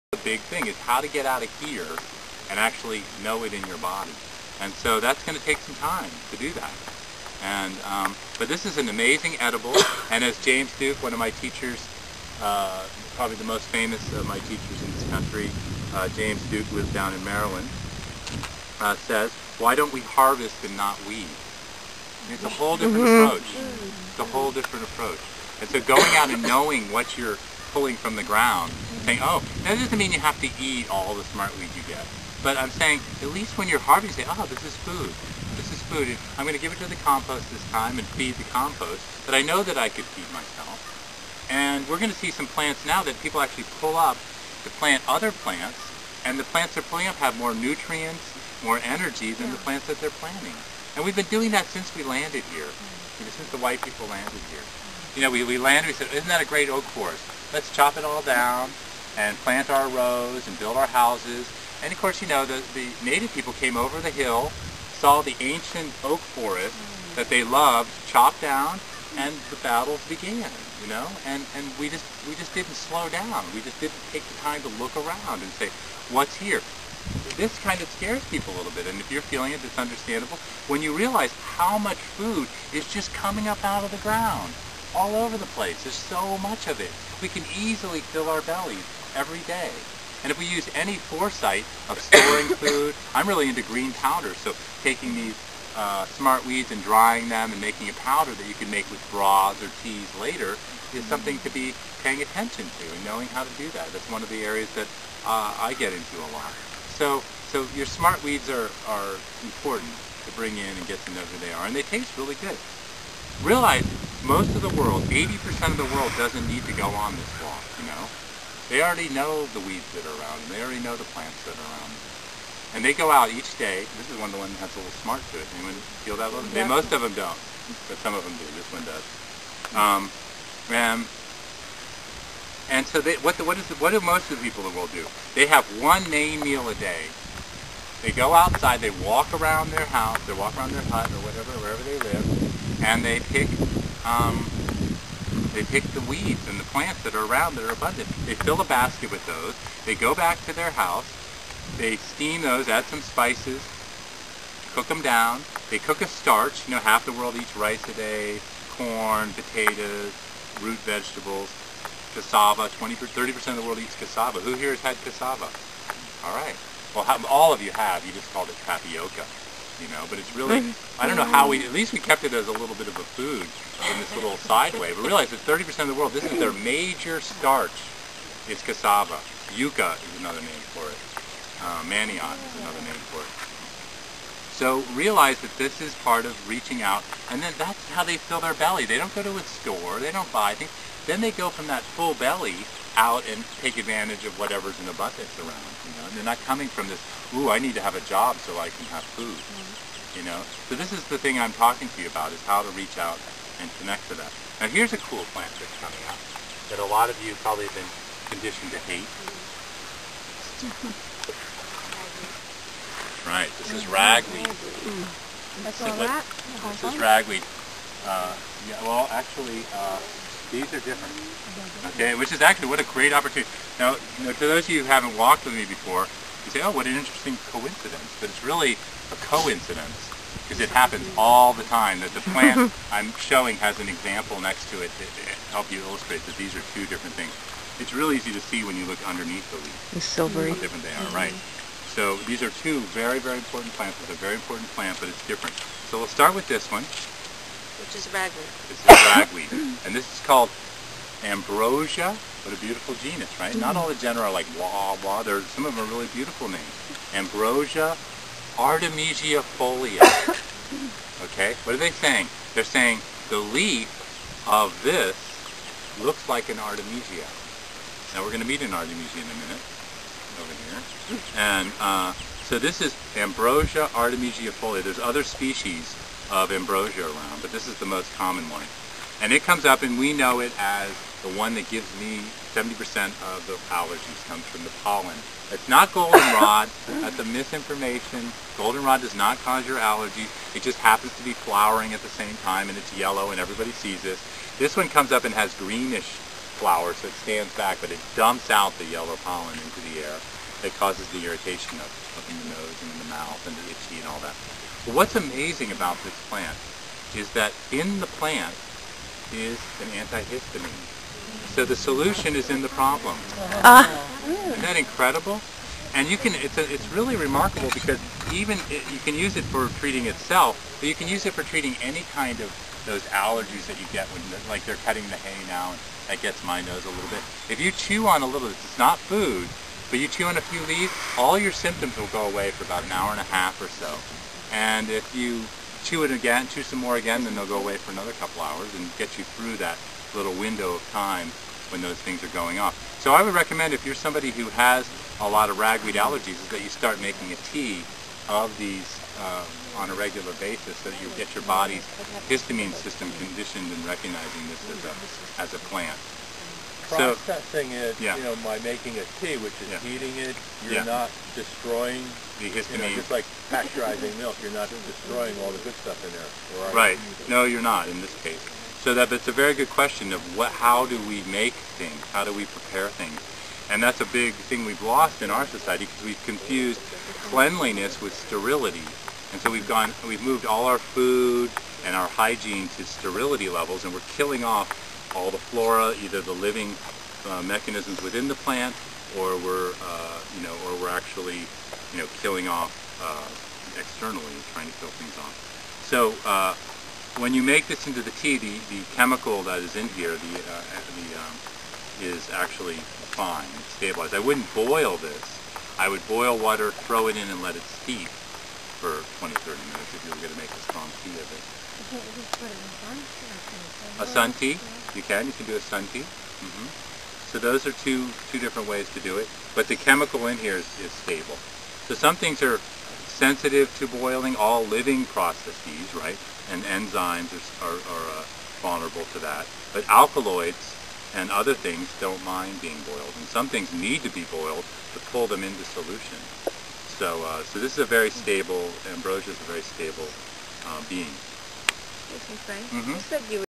Frank Cook: "Why Don't We Harvest Instead of Weed?"
Frank Cook, The last of the videos from his great plant walk Memorial Day 2009.
His website is His friends and family are working to make a documentary of his life.